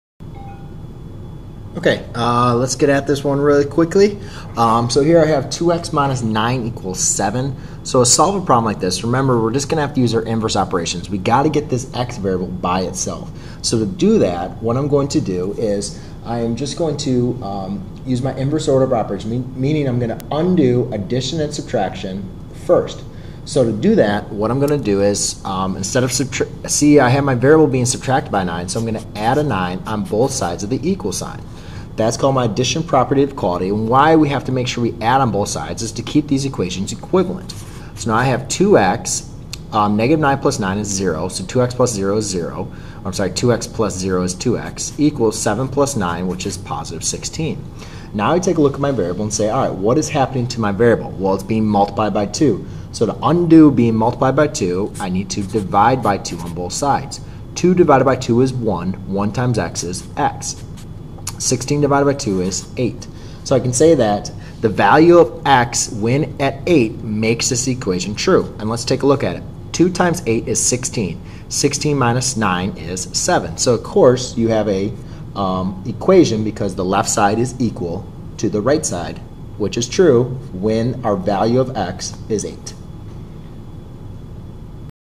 Solve a two step equation
👉 Learn how to solve two step linear equations. A linear equation is an equation whose highest exponent on its variable(s) is 1. To solve for a variable in a two step linear equation, we first isolate the variable by using inverse operations (addition or subtraction) to move like terms to different sides of the equality sign. After evaluating like terms we then also use inverse operations (multiplication or division) to remove any multiple to the variable.